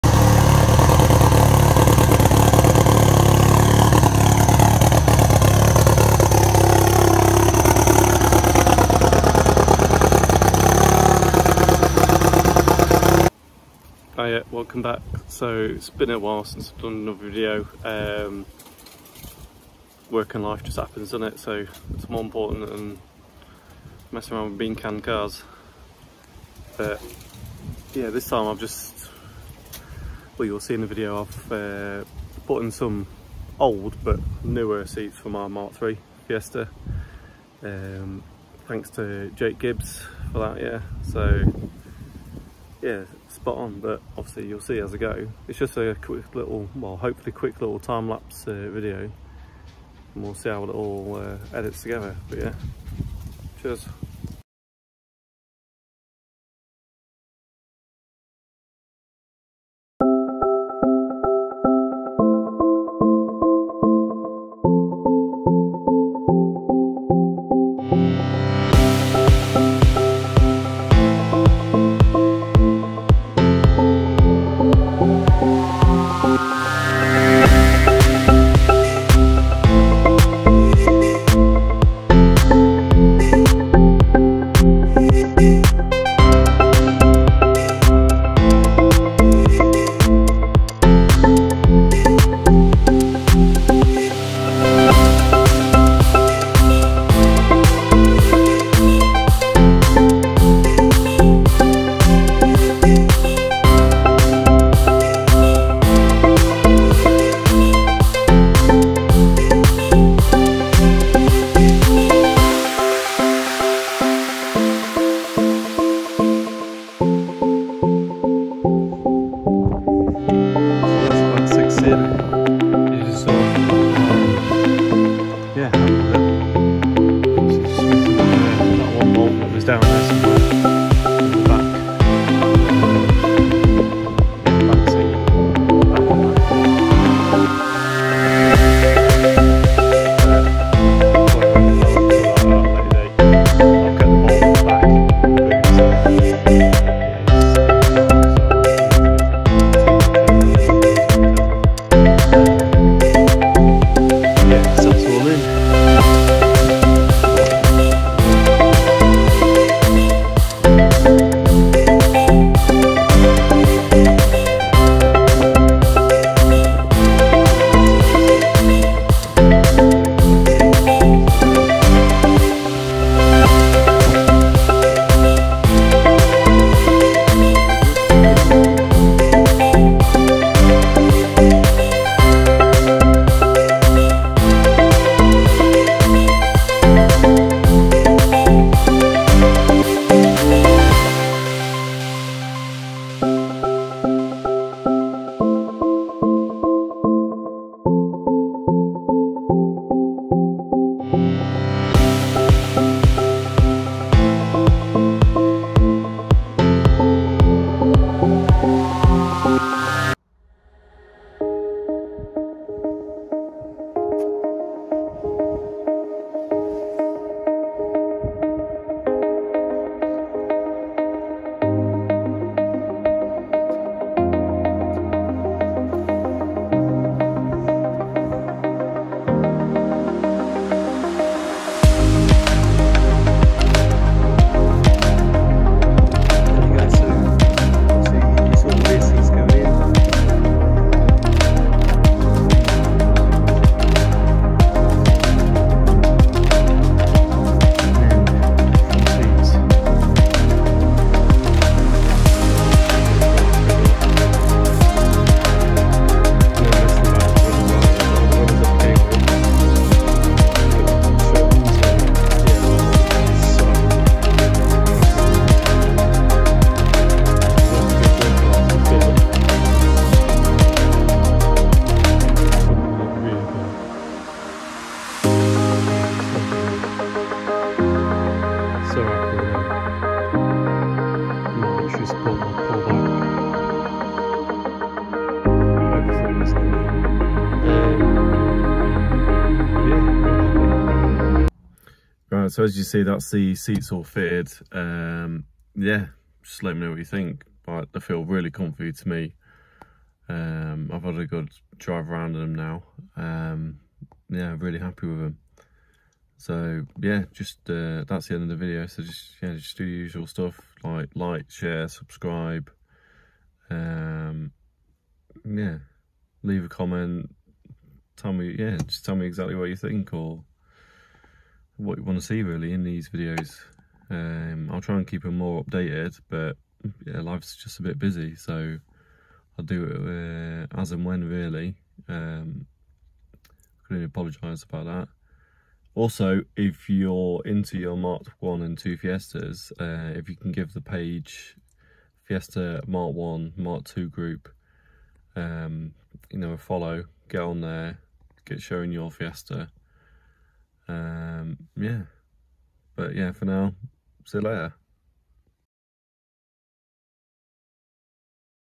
Episode 10, the daily gets newer seats
In this very delayed video I show you Mr Bean fitting some Si seats into my daily .